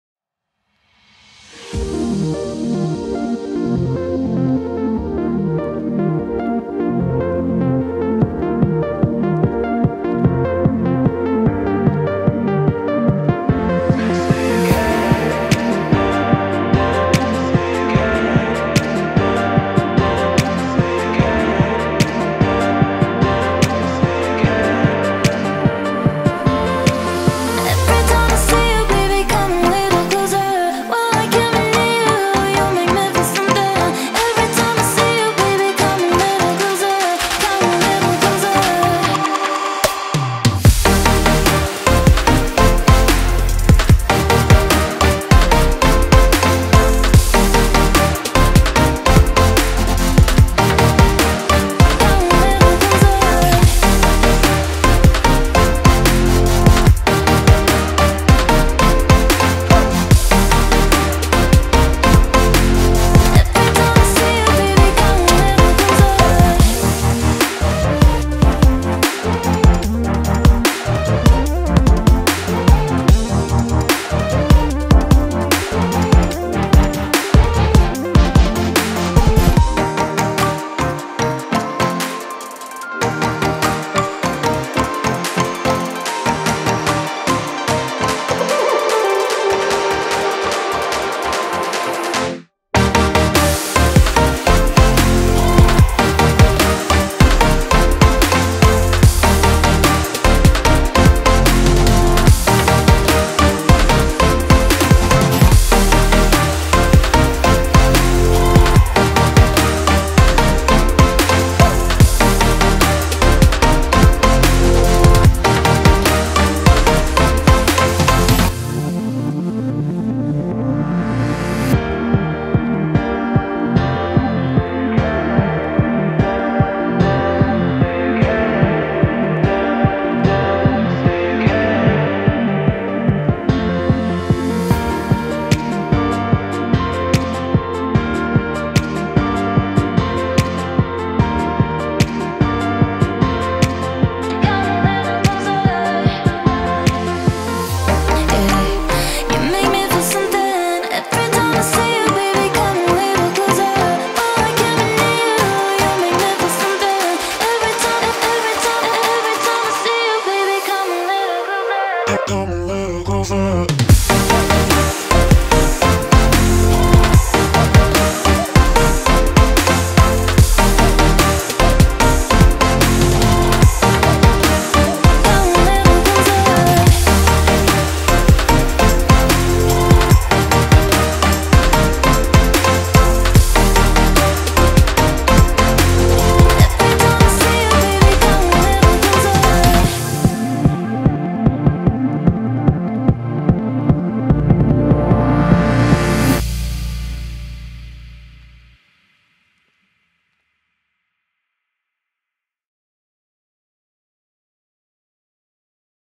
Southern Fall Day : GEPRC Smart35 HD
Cruising around on a hot fall day.  This crazy Alabama weather!